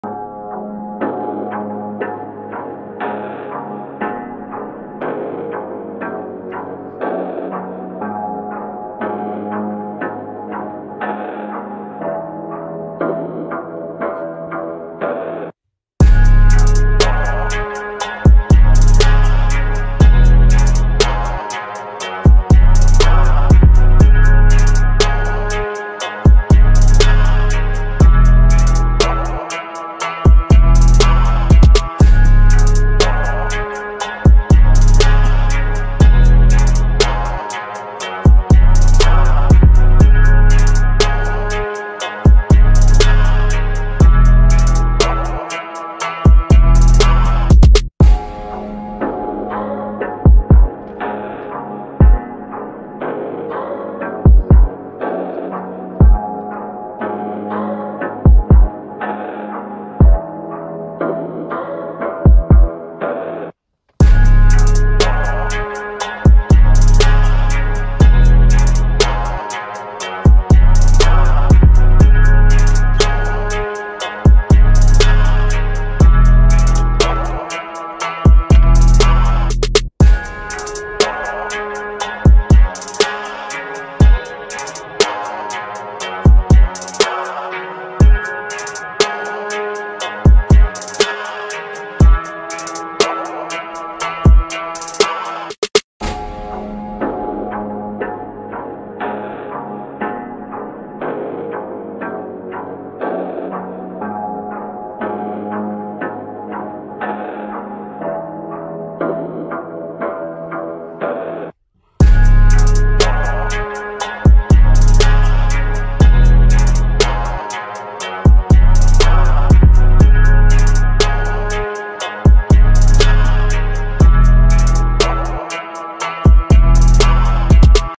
Dads when they see the grass getting long
Video by: Focal Point Media

Cameraman: Jake Hageman

Light and sound: Darian Hall

Makeup: Tyler Plasek

Dad: Kody Hughes

Busch light: Busch Light